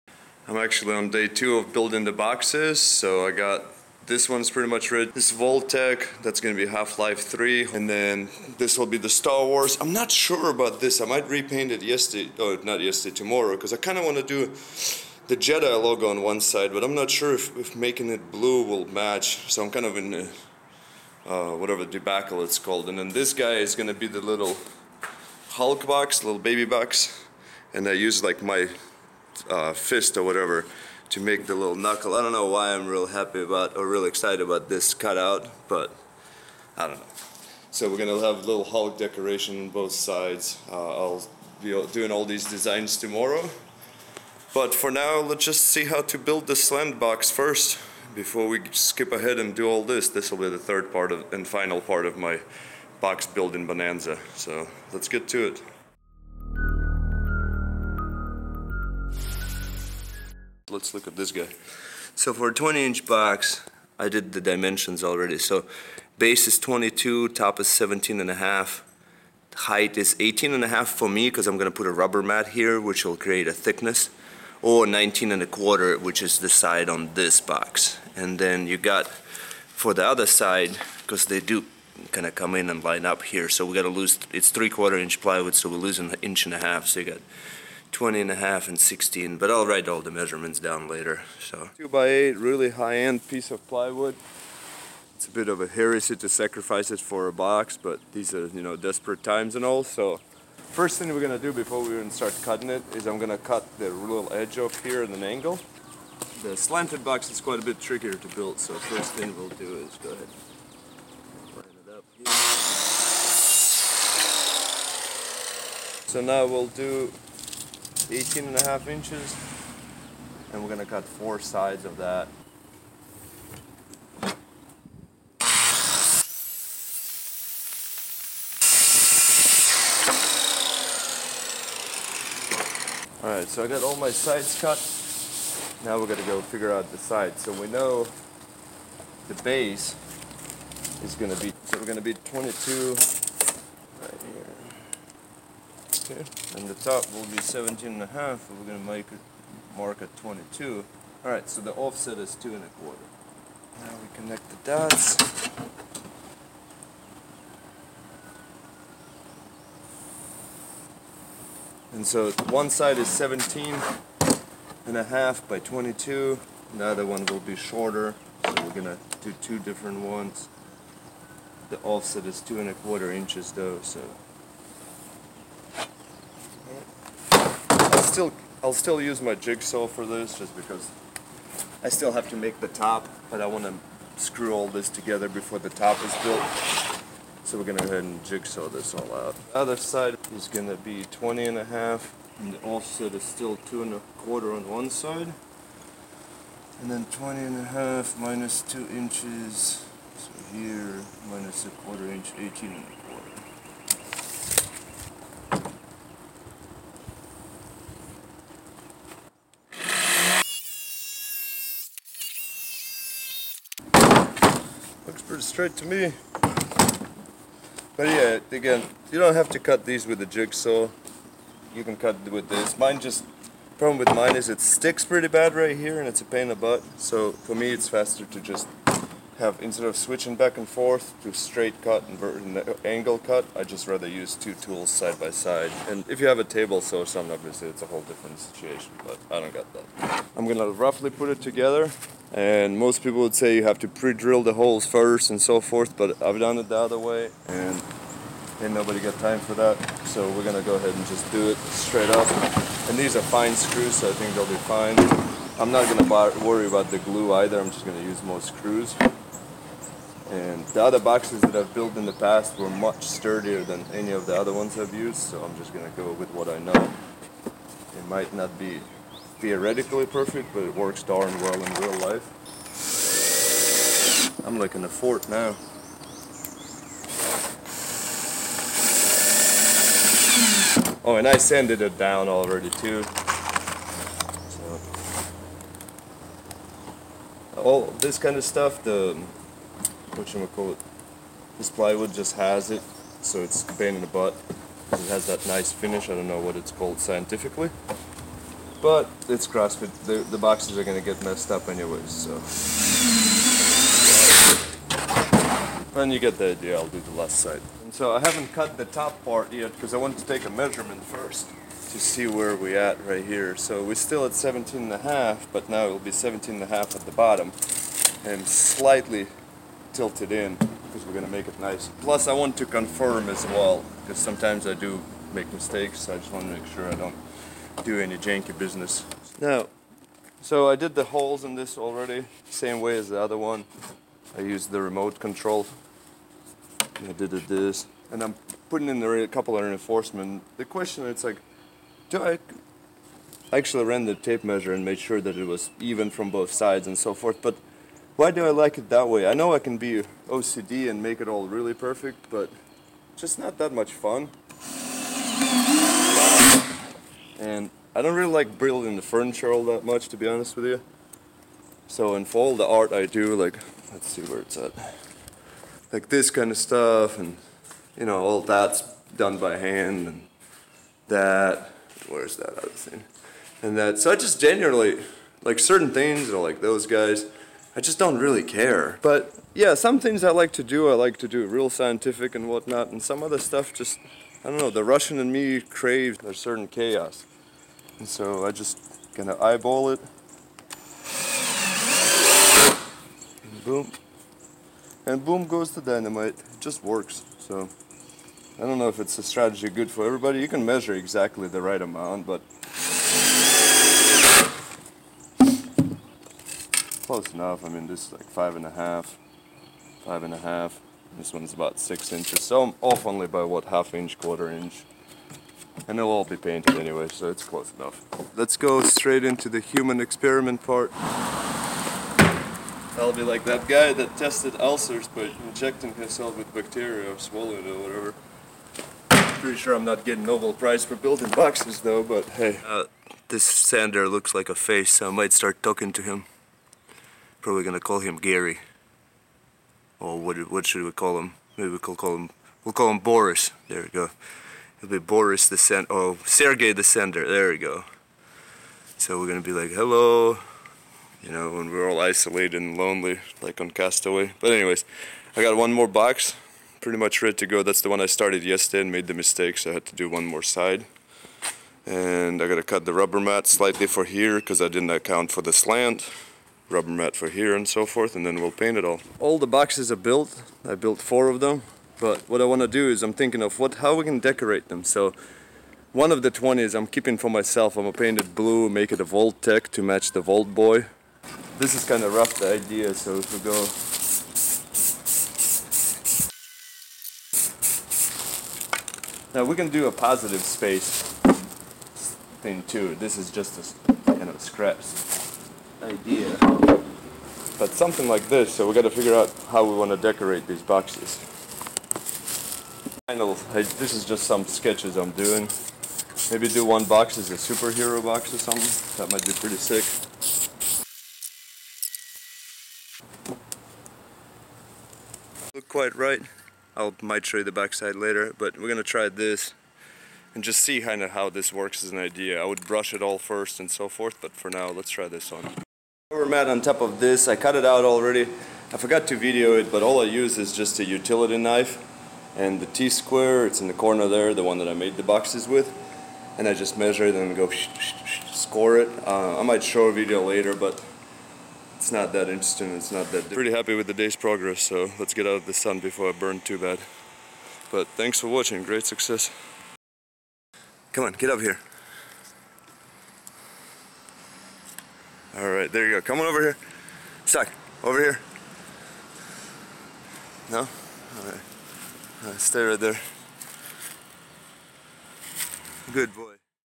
Making Plyoboxes Part 2: 20 inch Slanted Box
A little different from all the arts & crafts that I normally do - with almost 60% of gyms closed around the world (including mine - CrossFit Evolution) - I find myself with a little spare time (not much though so far).

We've given a lot of the gym equipment out & are running short on kettlebells, barbells, plates etc. But we're "Jigsaw Nation" and we have quite a stash of plywood (and toilet paper - but pls, don't share that).

So I figured might as well build some extra boxes (and decorate them). It's a fairly easy project to outfit a home gym as well.

In this 2nd video we're doing a slanted 20 inch box and starting to mock up some decorations a little bit. In the 3rd video the Pinteresting Man will strike & we're going to really go wild.

If you have any ideas of what we should make them as - post up! I'm going to sleep on some ideas & then make them into reality.

Superman & Star Wars stuff was just a mockup of what I had laying around.
Thumbnail is a teaser of what's coming in part 3.